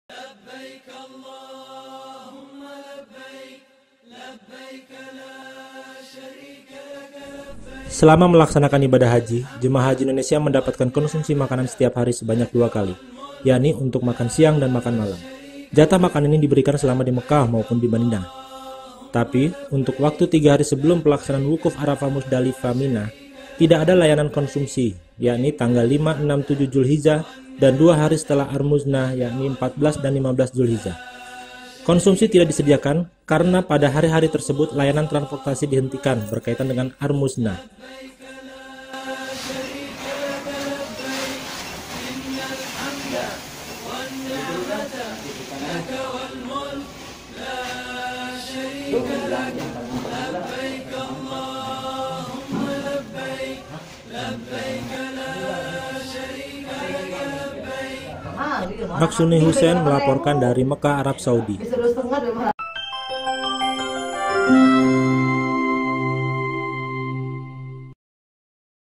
DISTRIBUSI DAN MENU JEMAAH HAJI
Selama melaksanakan ibadah haji, jemaah haji Indonesia mendapatkan konsumsi makanan setiap hari sebanyak dua kali. Yakni untuk  makan siang dan makan malam.

Jatah  makan ini diberikan selama di Mekah maupun di Madinah. Tapi untuk waktu tiga hari sebelum pelaksanaan wukuf Arafah Mudzalifah dan Mina (Armuzna) tidak ada layanan konsumsi, yakni tanggal 5, 6, 7 Dzulhijah  dan dua hari setelah Armuzna yakni 14 dan 15 Dzulhijah .

Konsumsi tidak disediakan karena pada hari-hari tersebut layanan transportasi dihentikan berkaitan dengan Armuzna.
Menu konsumsi yang disajikan dari ayam bakar, daging sapi hingga dadar telor.

Selain itu, ditambah menu sayuran seperti tumis, terong hingga jamur. Kemudian ditambah buah-buahan yakni pisang, buah pir, apel dan jeruk, dan air mineral. Untuk jatah makan malam ditambah satu buah roti untuk sarapan pagi.